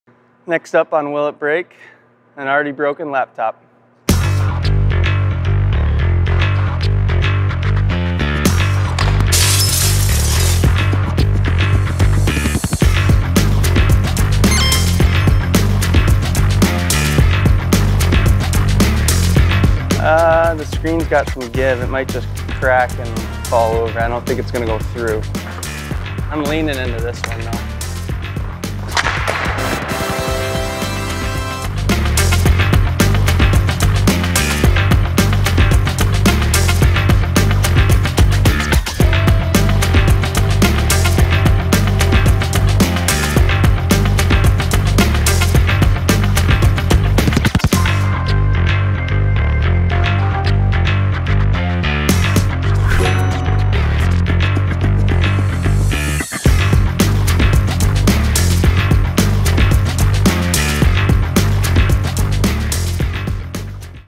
Will It Break? - Laptop
Jeff Petry puts his 102 mph slapshot to the test against a variety of items to see if they will break. In this episode: a laptop.